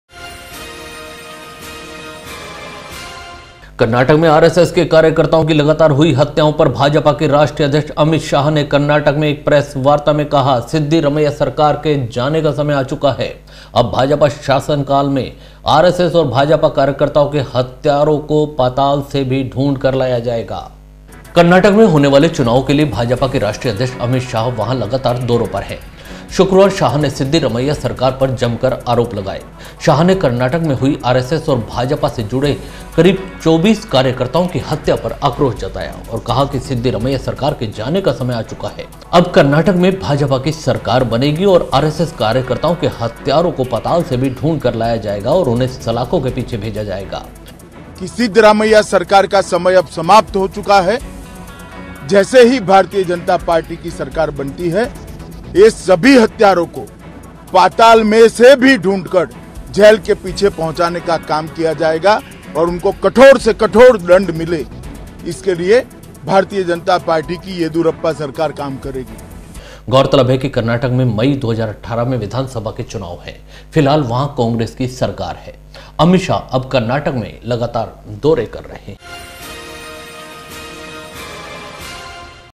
कर्नाटक में बनाएगी भाजपा सरकार ? अमित शाह ने सिद्धिरमैया सरकार पर लगाए गम्भीर आरोप
-कर्नाटक में आरएसएस के कार्यकर्ताओं की लगातार हुई हत्याओं पर भाजपा के राष्ट्रिय अध्यक्ष अमित शाह ने कर्नाटक में एक प्रेस वार्ता में कहा सिद्धिरमैया सरकार के जाने का समय आ गया है। अब भाजपा शासनकाल में आरएसएस और भाजपा कार्यकर्ताओं के हत्यारों को पाताल से भी ढूँढ कर लाया जायेगा।
 कर्नाटक में होने वाले चुनावो के लिए भाजपा के राष्ट्रिय अध्यक्ष अमित शाह वहां लगातार दौरों पर है। शुक्रवार शाह ने सिद्धिरमैया सरकार पर जमकर आरोप लगाये। शाह ने कर्नाटक में हुई आरएसएस और भाजपा से जुड़े 24 कार्यकर्ताओं की हत्या पर आक्रोश जताया और कहा सिद्धारमैया सरकार के जाने का समय आ गया है, अब कर्नाटक से भाजपा की सरकार बनेगी और आरएसएस कार्यकर्ताओं के हत्यारों को पाताल से भी ढूँढ कर लाया जायेगा।
 गौरतलब है कि कर्नाटक में मई 2018 में विधानसभा चुनाव हैं, फि़लहाल वहां कांग्रेस सरकार है। अमित शाह अब कर्नाटक में लगातार दौरे कर रहे हैं।